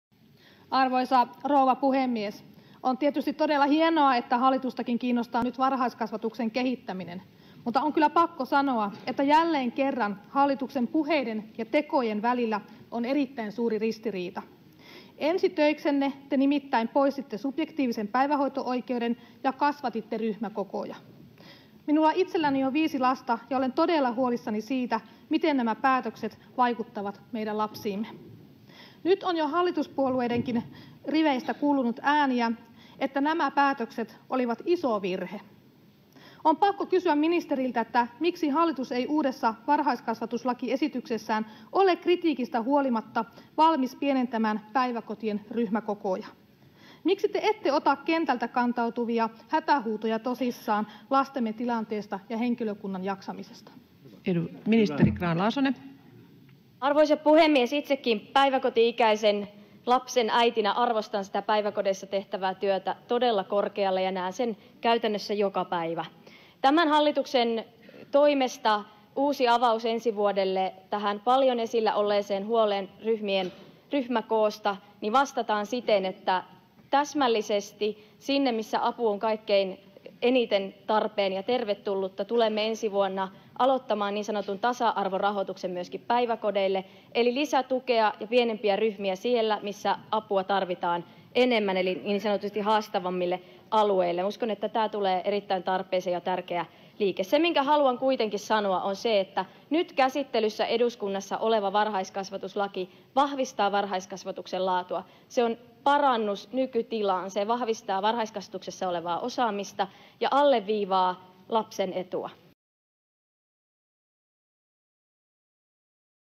Pienempiä ryhmäkokoja!
Sipilän hallitus on poistanut lapsilta subjektiivisen oikeuden päivähoitoon ja kasvattanut ryhmäkokoja, mikä rasittaa sekä lapsia että henkilöstöä. Kansanedustajamme Katja Hänninen kysyikin ministeri Grahn-Laasoselta, miksi eduskunnan käsittelyssä oleva esitys varhaiskasvatuslaiksi ei puutu ongelmiin, vaikka päätöksiä on kritisoitua hallituspuolueita myöten.

-Miksi te ette ota kentältä kuuluvia hätähuutoja tosissaan lastemme tilanteesta ja henkilökunnan jaksamisesta?